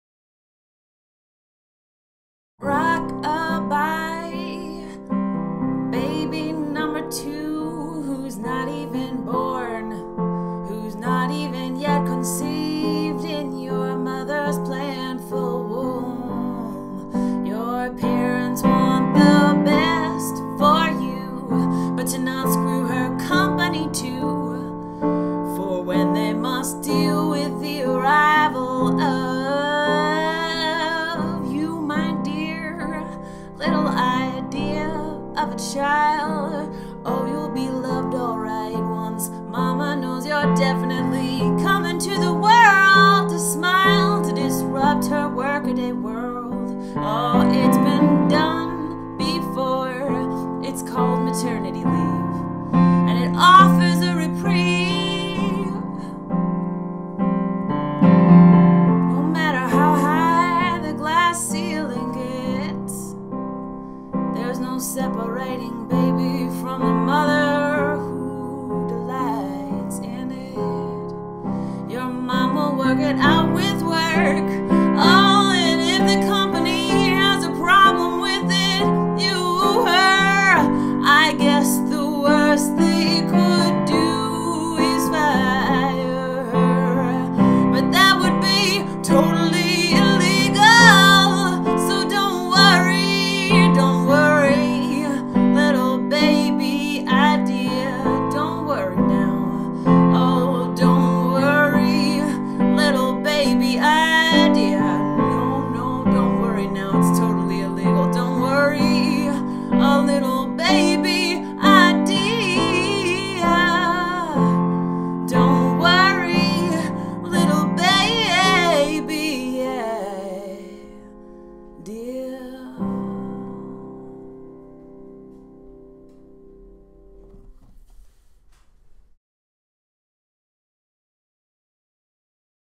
Advice Music vol 73
RZ advises a working mom but addresses the real idea in question.